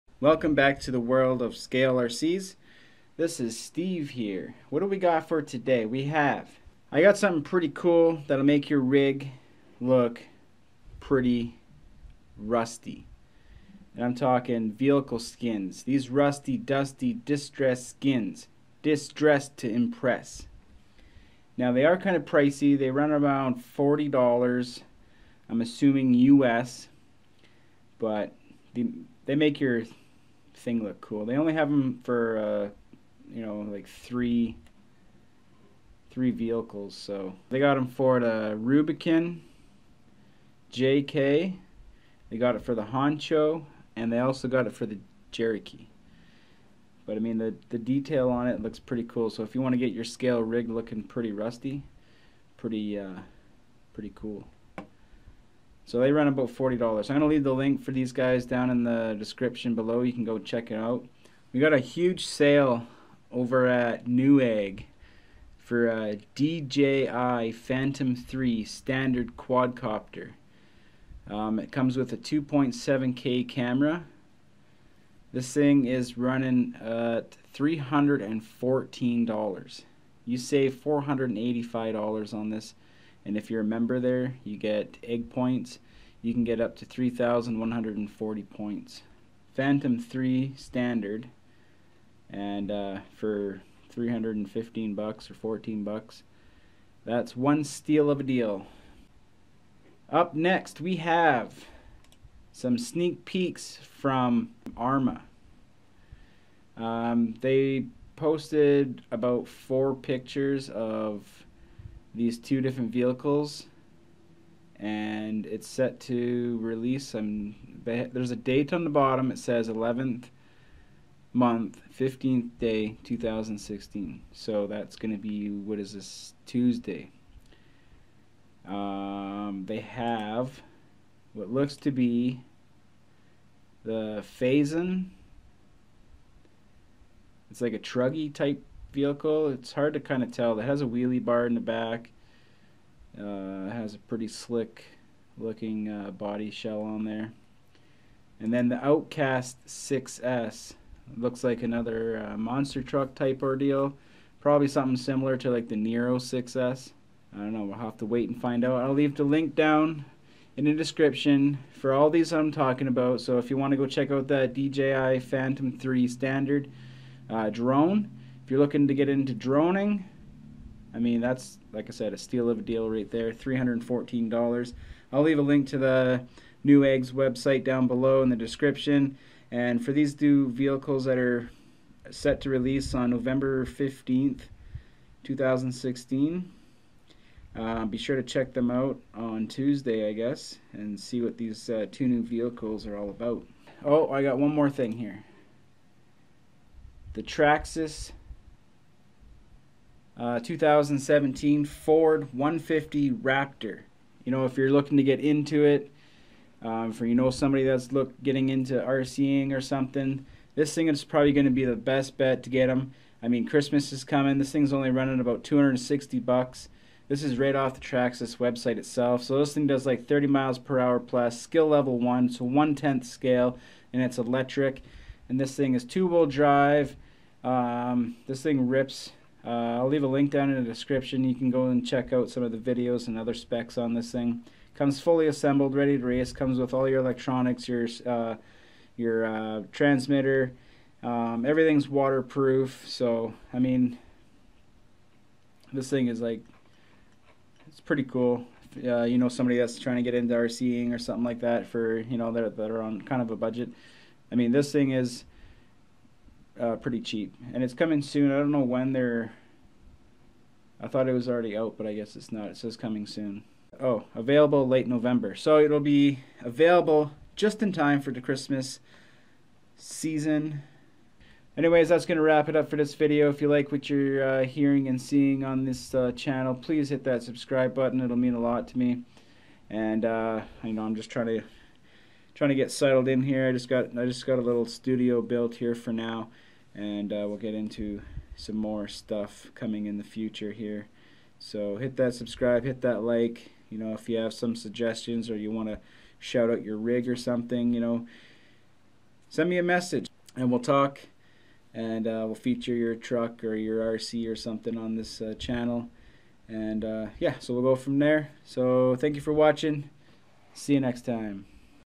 New Releases From Arrma and Traxxas Rusty Skins Cheap DJI 3 Drone
Traxxas is set to release the Ford 150 Raptor. 1/10 Scale electric 2wd truck in late November. Arrma posted 4 pictures of 2 different RC's that look to be released on Nov. 15 2016 and Newegg has a Phantom 3 DJI Drone for $314.00 Super Cheap and a steal of a deal. It is refurbished but still a great deal. Also Rust bucket skins are available for the Axial Cherokee, Axial Honcho, and Rubicon JK bodies.